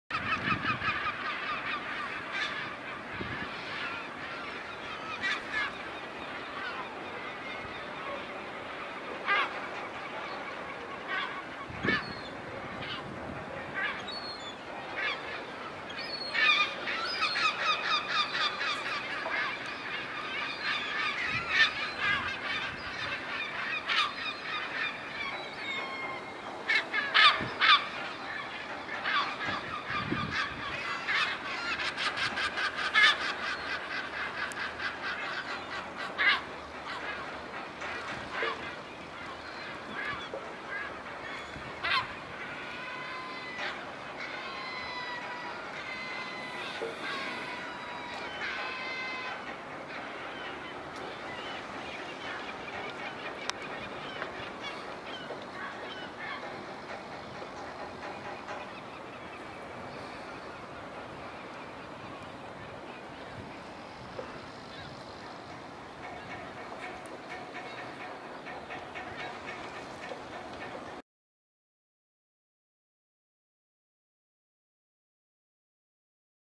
Morning animal life in the harbor of Coquimbo in Chile
Morgendliches Tierleben am Hafen von Coquimbo in Chile.